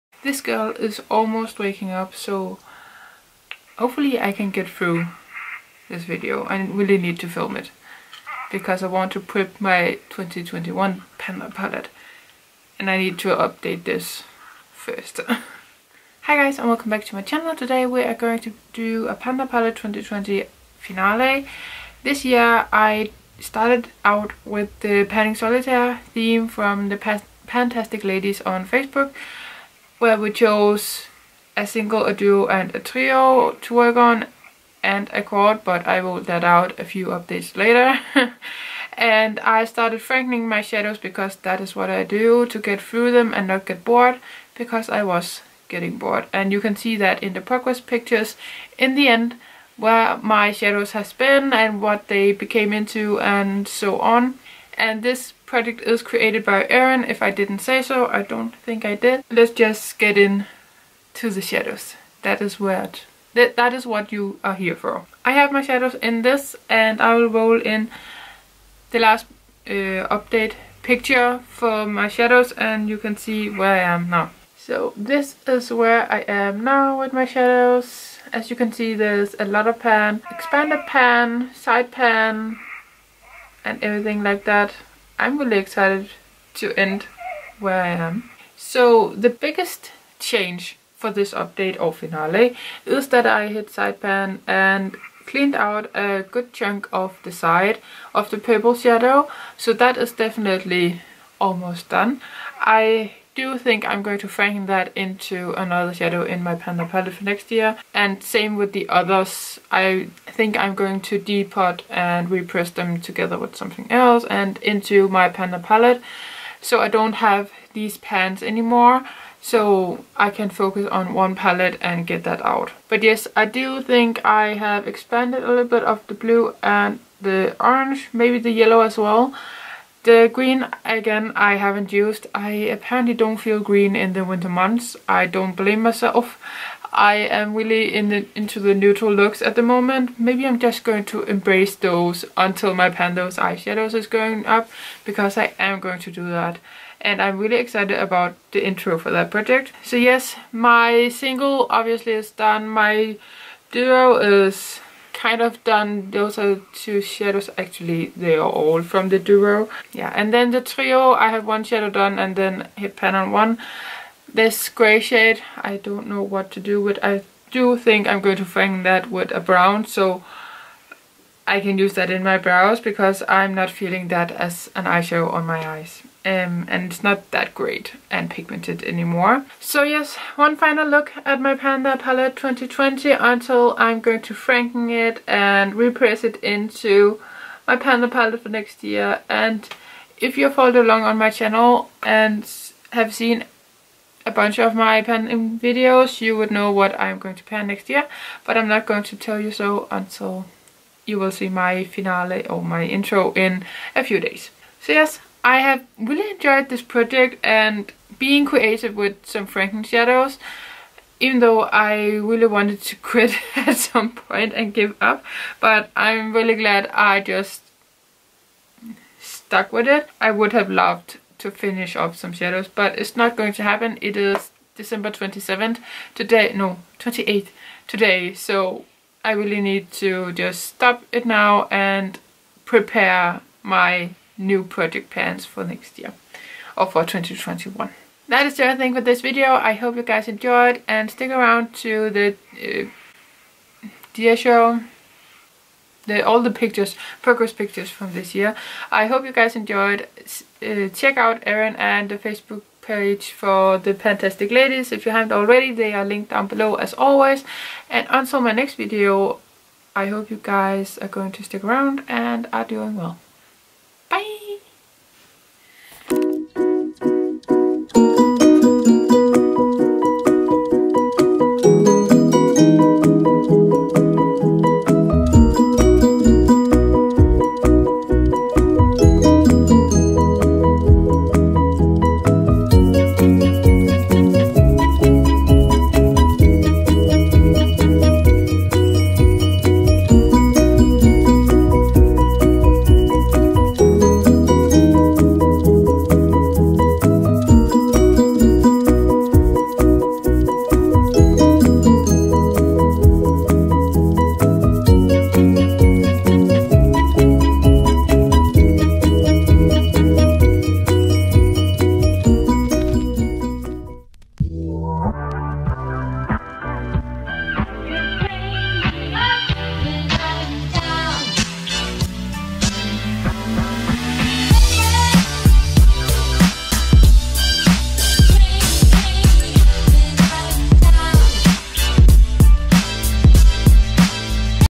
PAN THAT PALETTE 2020 | FINALE // COLLAB
I have decided to join the PanTasticLadies yearly PTP! This year the theme is based on Solitare.
This is a progression type of pan that palette. You start with a single, then move to a duo, trio, quad, etc. Your shadows can be in a palette, single, depotted, etc. and can be panned either rolling or simultaneously. The end goal is to pan as many shadows and "sets" as you can.

My "Sets"

Single: Benefit - Call me buff (DONE)
Duo: Benefit - Taupe it off & Nude swings (1/2 DONE)
Trio: Catrice - On the taupe of the Matt everest, Starlight Espresso & Game of stones (1/3 DONE)